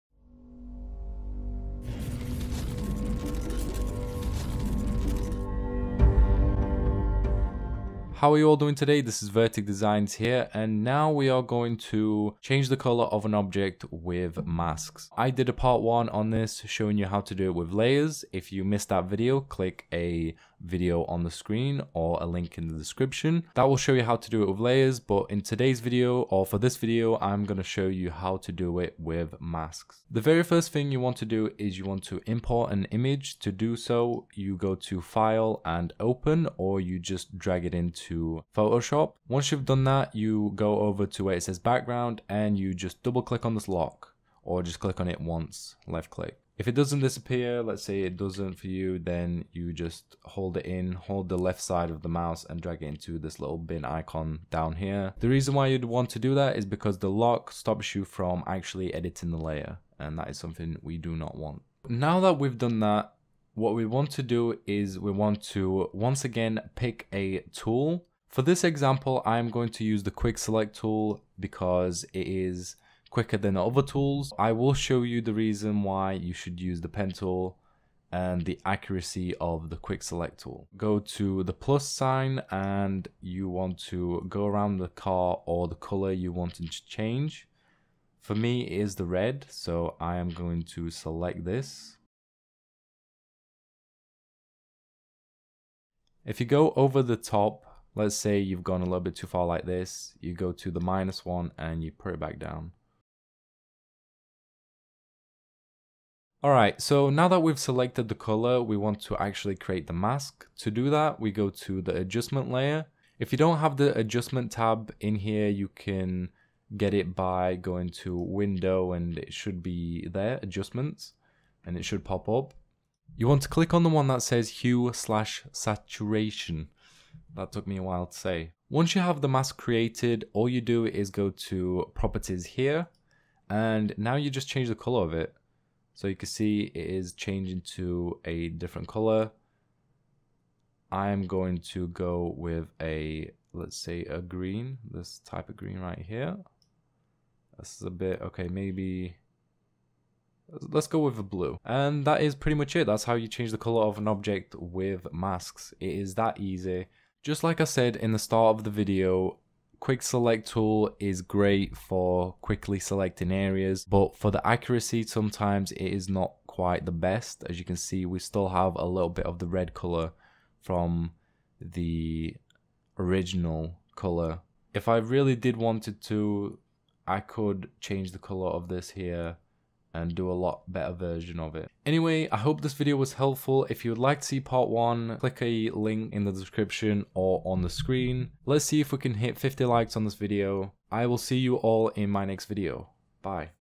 Photoshop How To Change Color of Object - With Masks Part 2 (Adobe Photoshop CS6/CC Tutorial)
Adobe Photoshop cc is a wonderful program full of different tools. To change the colour of an object with masks it's easy to do. I will tell you how to do it in this adobe Photoshop cc tutorial. Masks are similar to layers but they work differently and can give similar results. It's all about what you prefer.

Having problems with Editing, Photoshop or YouTube?